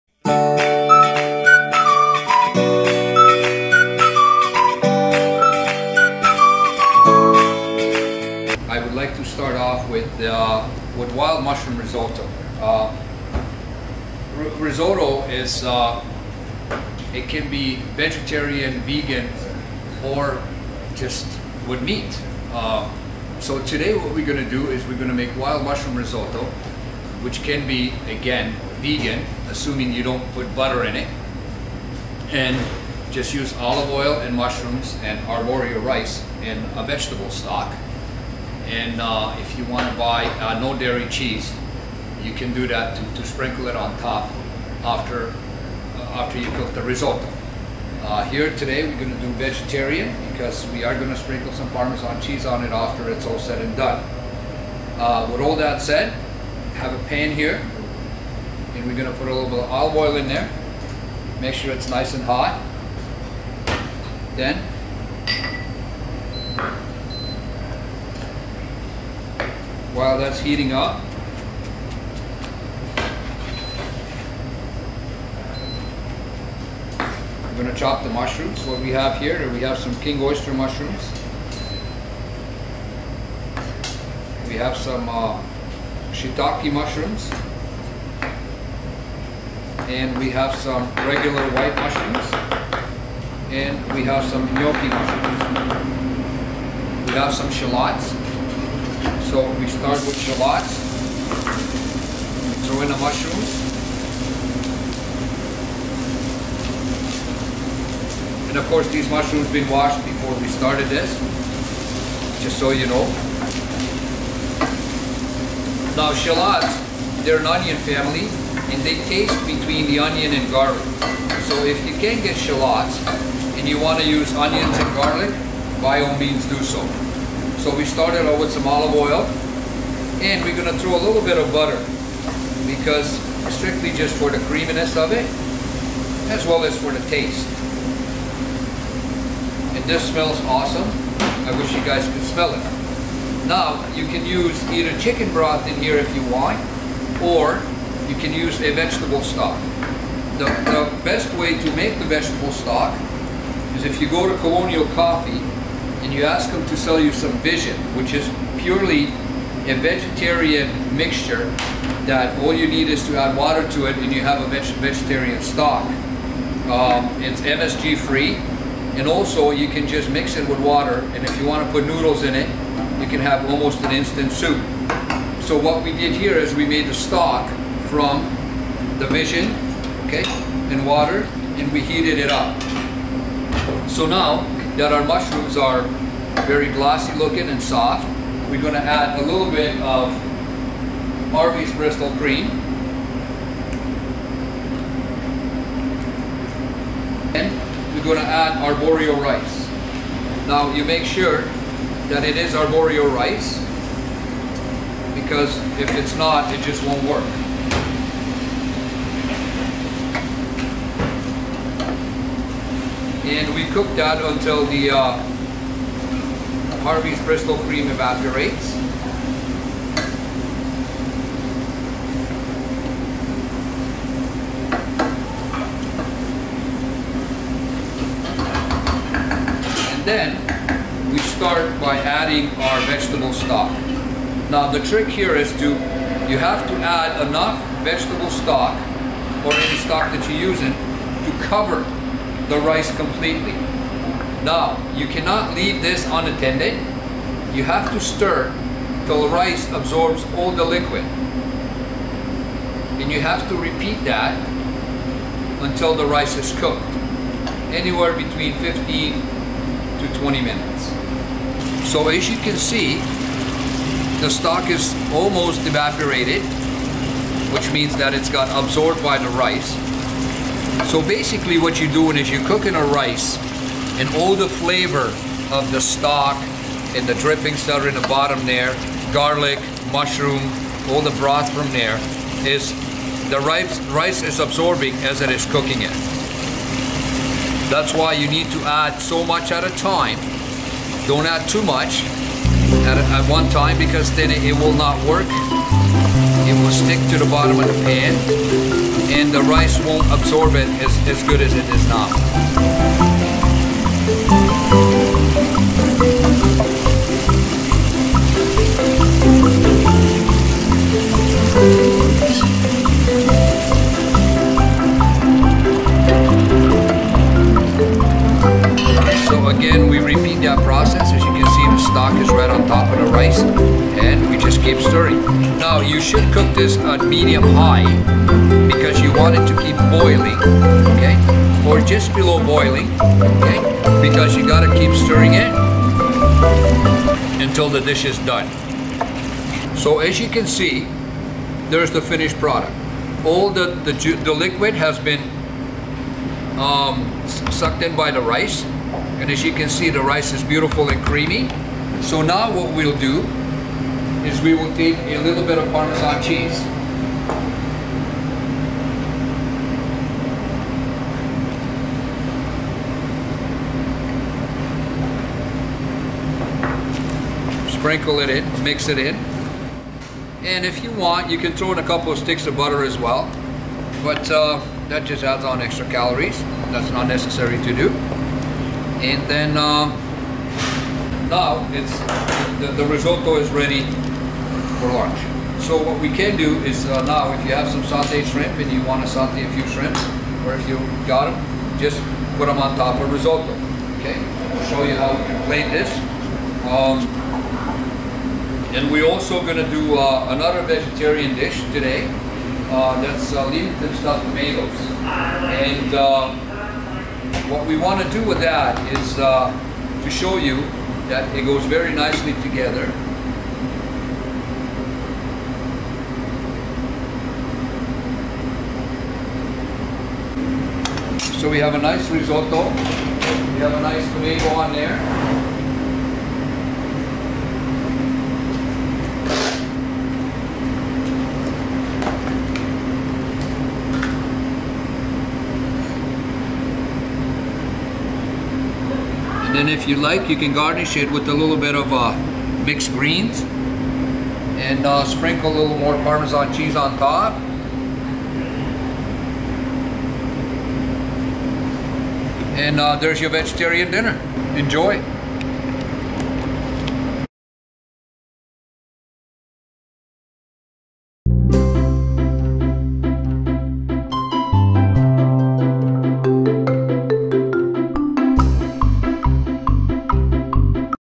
Wild Mushroom Risotto Chef John
Learn to make Wild Mushroom Risotto with Chef John Kukucka Executive Chef of Essex Golf & Country Club. In this video Chef John explains how to make this dish completely vegan or vegetarian. Enjoy!
Wild mushroom Risotto

1 L vegetable broth from vison
3 tbsp butter
3 tbsp olive oil
2 shallots or 1 medium onion, diced
1 lb of wild mushrooms such as (oyster, cremini, shiitake) sliced 2 cup Arborio rice
½ cup sherry
½ cup parmesan cheese
¾ tsp of fresh diced thyme

Make vegetable broth and simmer on stove
Melt butter in a heavy bottomed sauce pan over medium heat, add shallots or onion and saute for 1 minute, add sliced mushrooms and fresh thyme, saute until mushrooms are tender and juices are released, add rice and saute for I more minute, stirring constantly Add sherry and stir until liquid is absorbed,  stirring frequently about 3 minutes
Increase heat to medium high, add 1 cup vegetable broth and simmer, you will have to keep adding broth as it absorbs, stir until it becomes creamy about 20 minutes,
Take off heat and mix in parmesan cheese